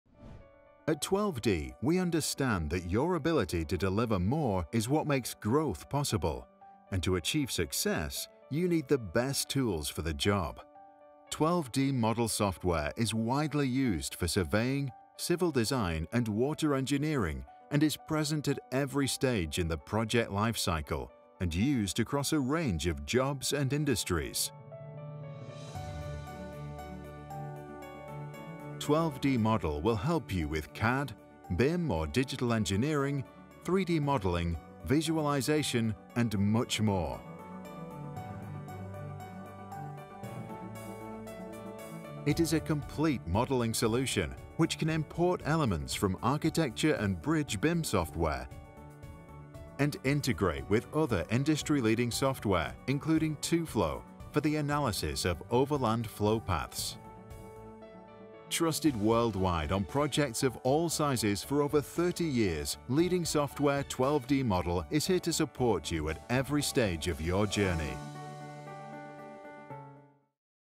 12d Model Software - A Brief Overview
Did you know that 12d Model is a single software application that covers surveying, civil, and water engineering? Reduce the complexity of studying multiple products, and achieve more with less. See all the different ways you can use 12d Model today!